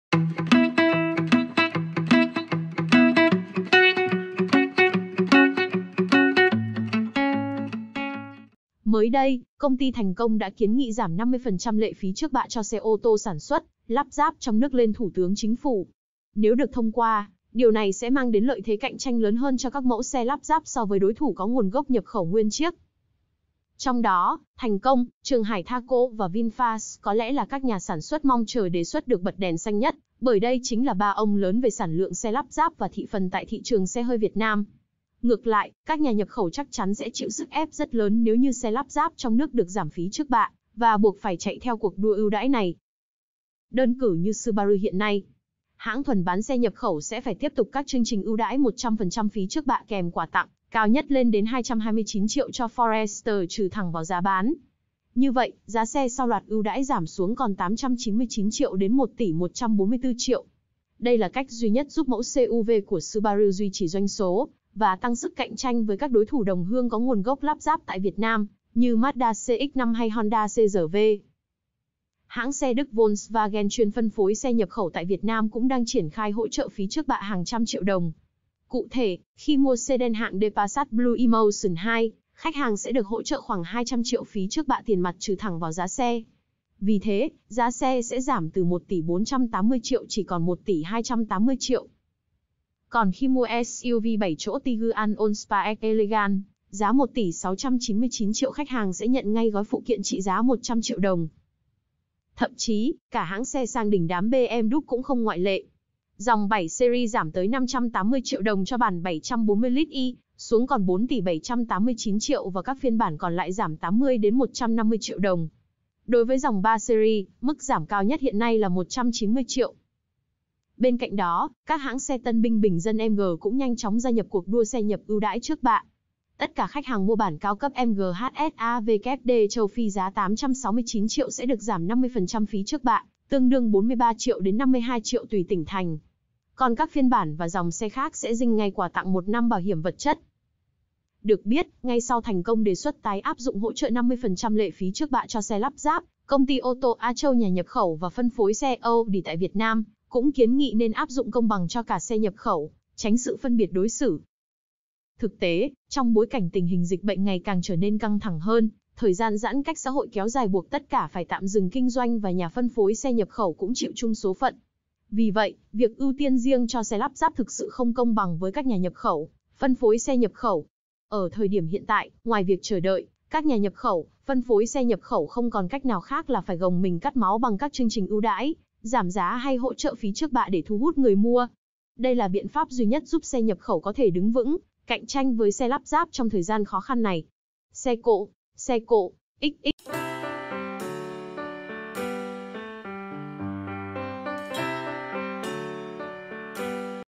Hết đường đành tự "cứu" mình xe nhập khẩu tự “cắt máu” ưu đãi trước bạ trên 100 triệu cho khách Việt
Nhiều xe nhập khẩu đang phải “cắt máu” khi tung ưu đãi trước bạ lên đến cả trăm triệu đồng cho khách hàng Việt. Đây được xem là động thái tự cứu lấy mình trước đề xuất giảm 50% lệ phí trước bạ cho xe ô tô sản xuất, lắp ráp trong nước.